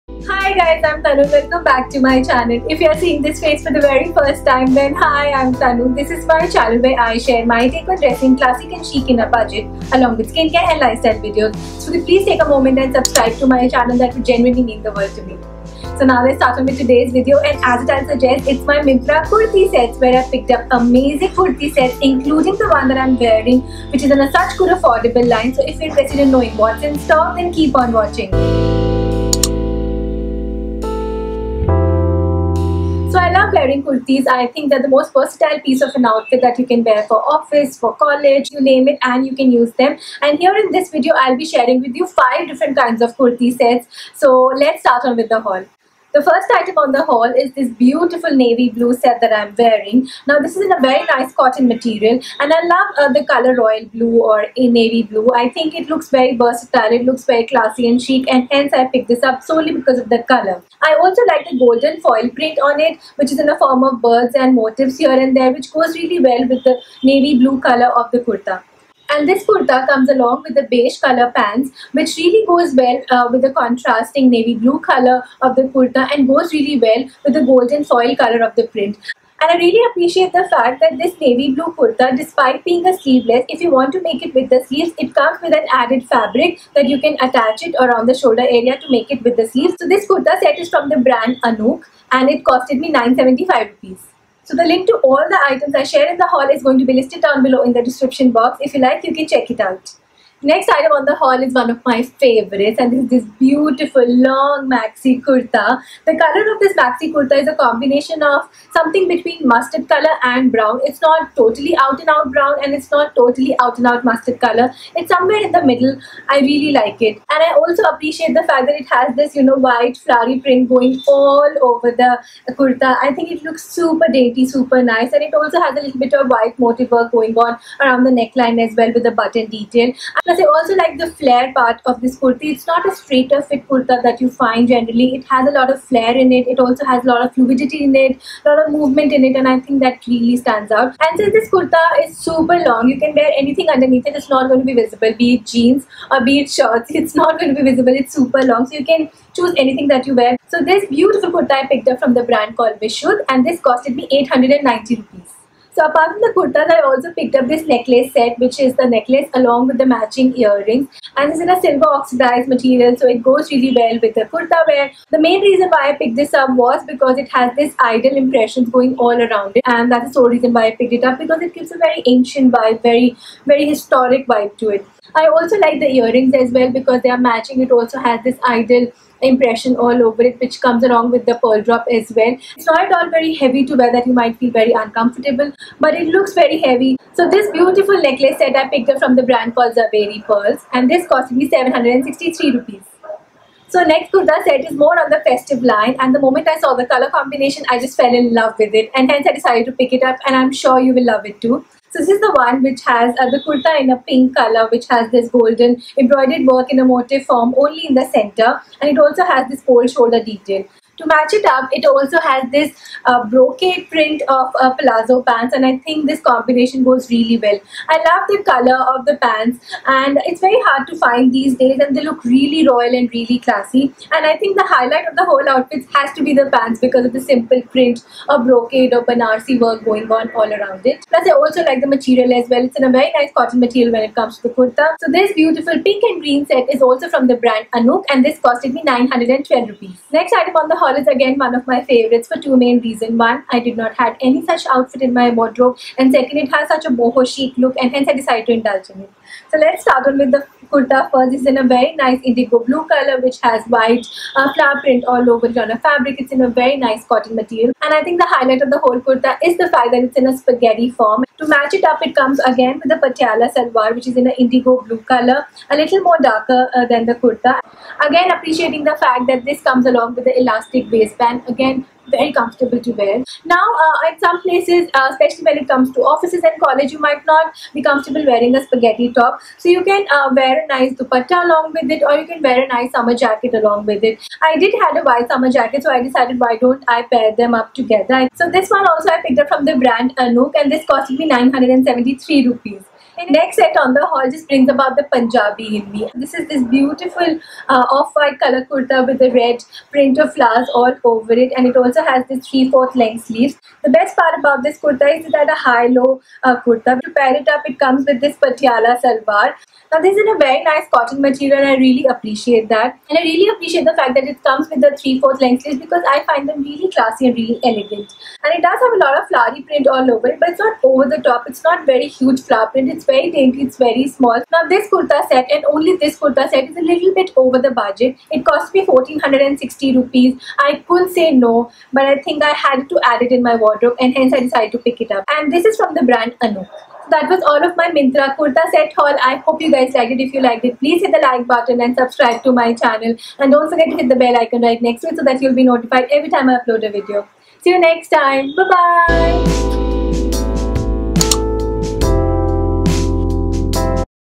MYNTRA KURTI HAUL | AFFORDABLE KURTI SET HAUL | Tanu Gupta
Hey Cuties,

Here are my 5 different types of KURTI SET haul , all from myntra. Hope you guys like it .

- Blue & Beige Kurti Set , Size S , Rs. 975
- Pink & Green Kurti Set, Size S, Rs. 912
- Indigo Blue Kurti Set , Size S,Rs. 973
- Red Kurti Set, Size S, Rs. 1349

xoxo
Tanu Gupta